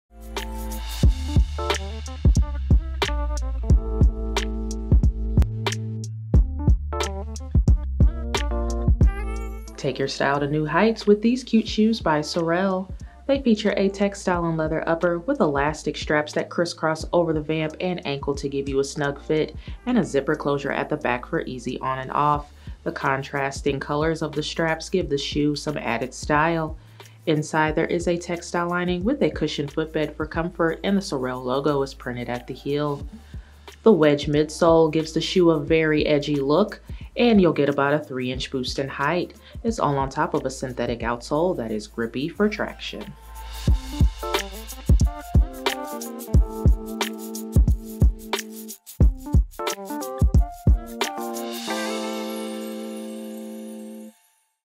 SOREL Joanie™ III Sport Strap SKU: 9598409
Bring a sporty style to your warm-weather wardrobe with the SOREL&reg; Joanie&#8482; III Sport Strap sandal.
Slip-on design features a leather and textile upper which features criss-cross straps across the ankle and vamp. Single leather adds an additional secure fit across the foot.
Zip-up closure at the back of the heel.
Rounded, open-toe design.
Textile lining and insole offer shock absorption and  comfortable underfoot support.
Molded rubber outsole with platform height and pronounced lugs for added traction and durable wear.
Imported.